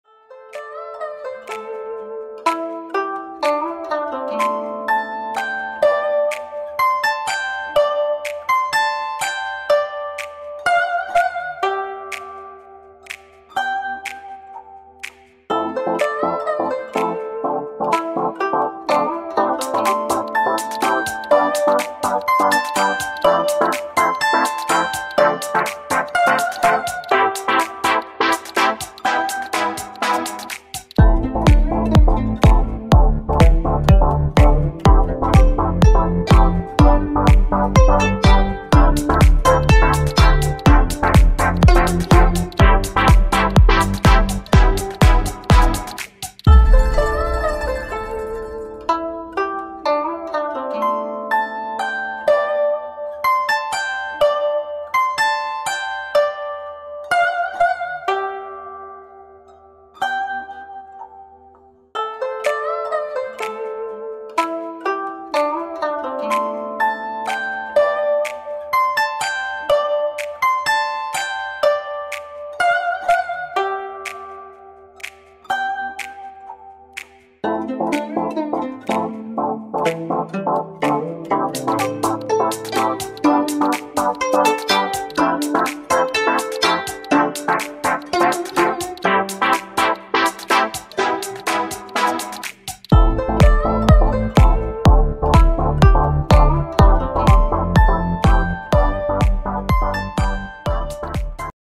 Pug dog go outside
Hello guys I’m back ..today I will go with my daddy for walk 😍😍